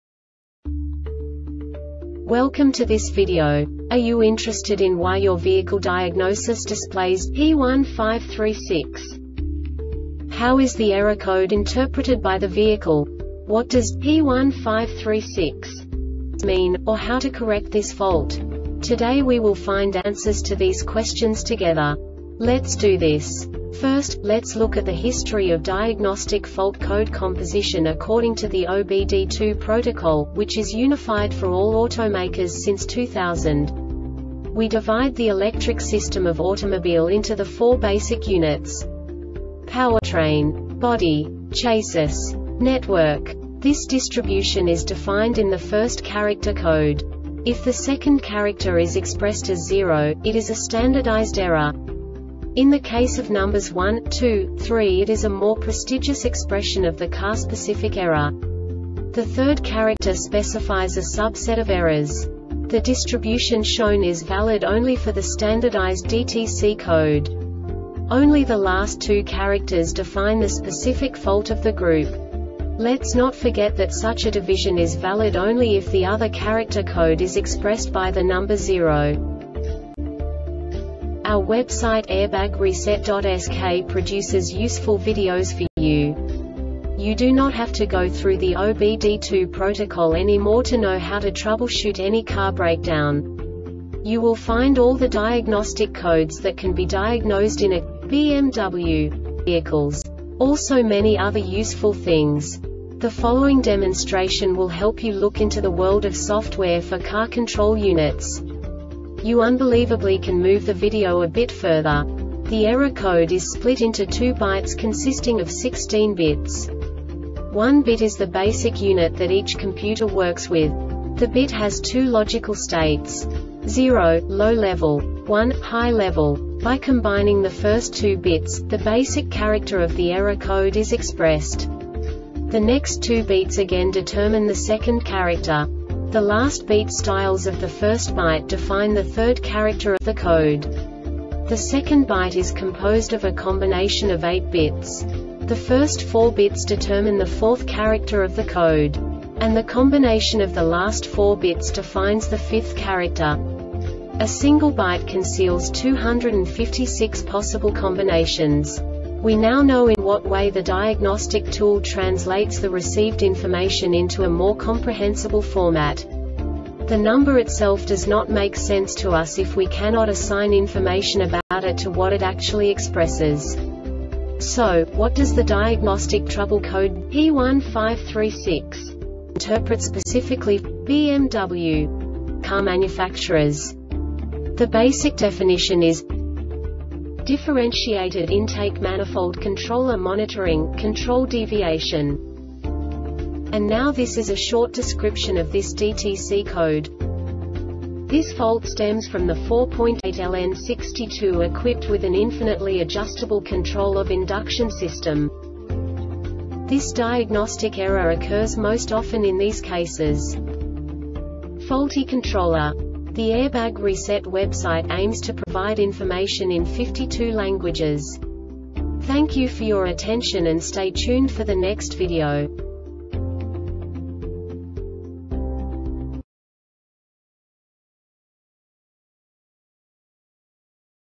DTC BMW P1536 Short Explanation
Contents:
0:21 Basic DTC analysis according to OBD2 protocol standard.
1:48 Insight into programming
2:58 A diagnostically understandable description of the Diagnostic Trouble Code
3:05 Basic error definition

Make:
BMW
Code:
P1536
Definition:
Differentiated Intake Manifold Controller Monitoring, Control Deviation
Description:
This fault stems from the 4.8L-N62 equipped with an infinitely adjustable control of induction system.
Cause:
Faulty controller